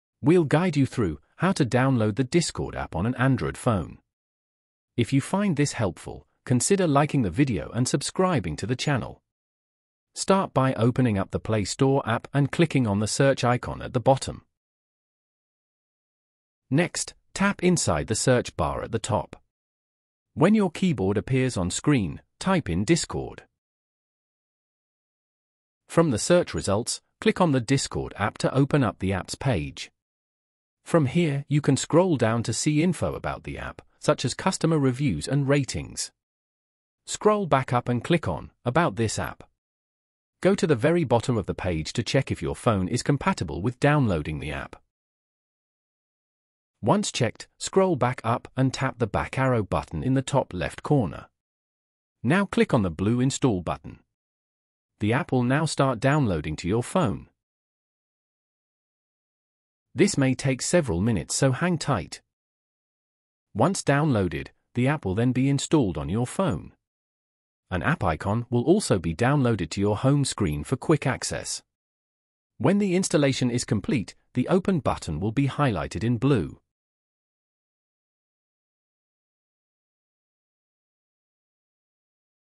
How To Download Discord App On Android Phone (Step By Step)
This video will show you how to download the Discord app on an Android phone.